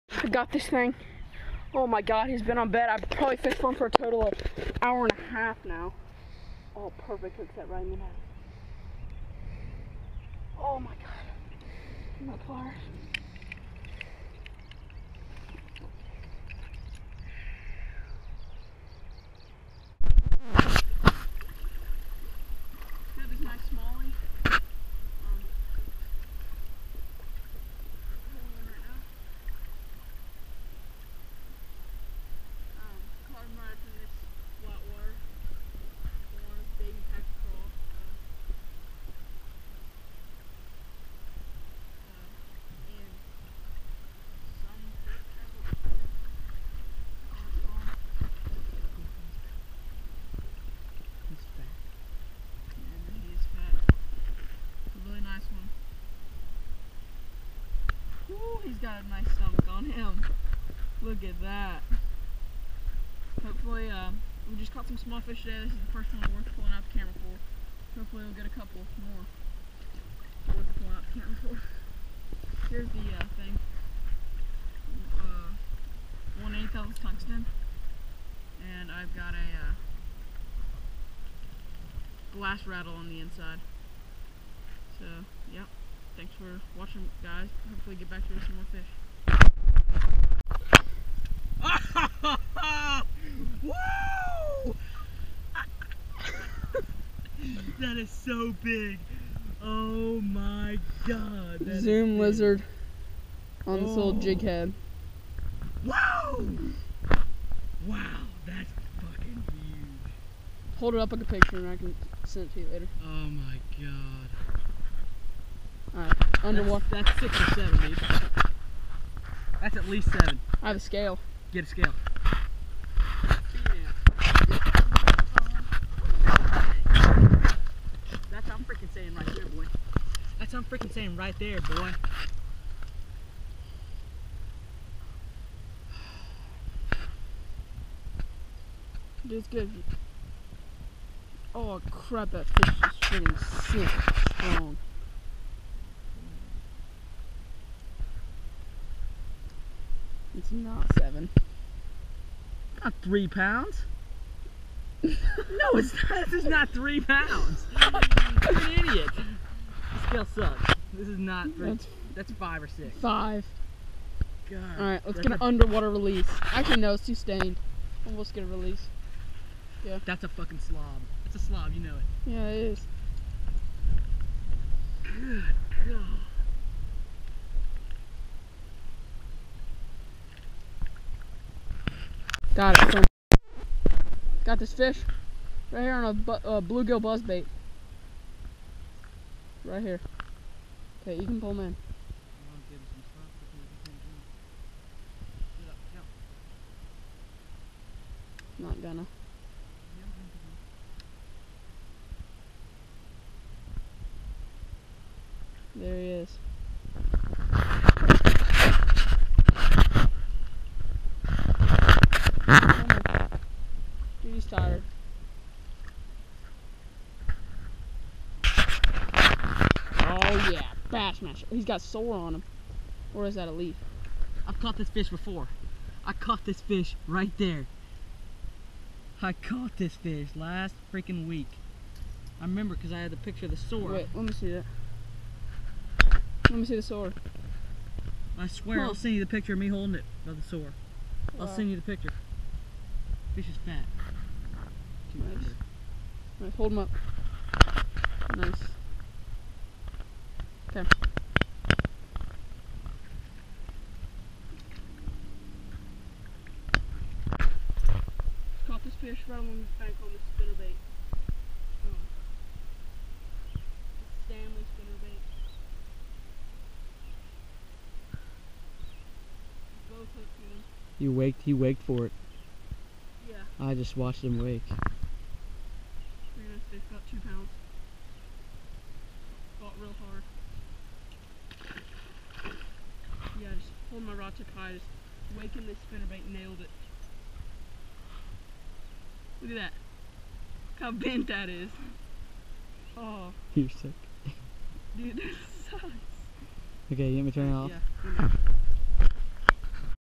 Spinnerbait/Buzzbait Spring Bass
We fished two ponds at this golf course and in that day we caught apron. 30 bass but these and many be 1 or 2 more were the ones of any size